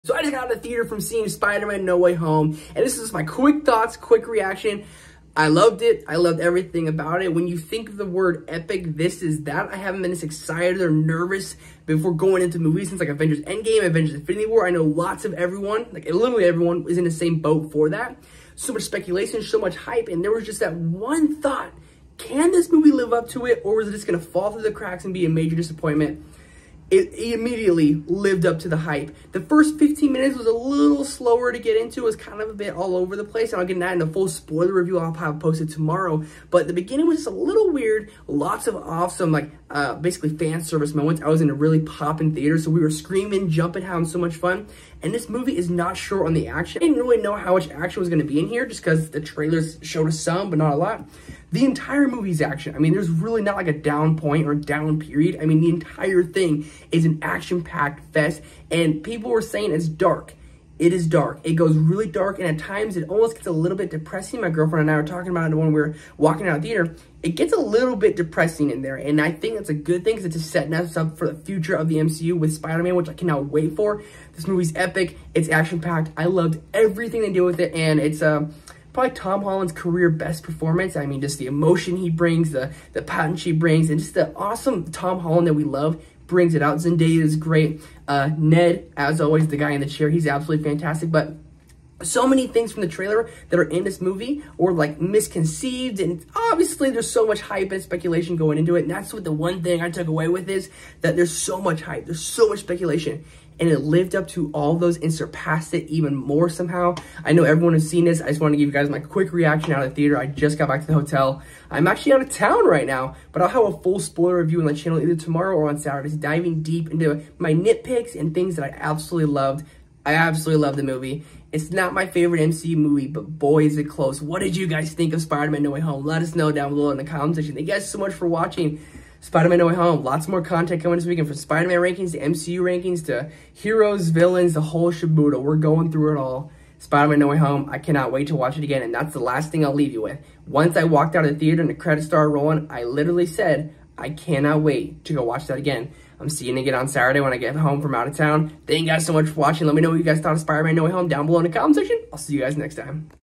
Spider-Man No Way Home Out Of The Theater REACTION! (BEST MCU FILM?!)
SpidermanNoWayHome is absolutely perfect. I loved every single thing it has to offer. Cannot wait to see it again!

This is my quick thoughts right after seeing the movie!

Full Spoiler Review coming Saturday!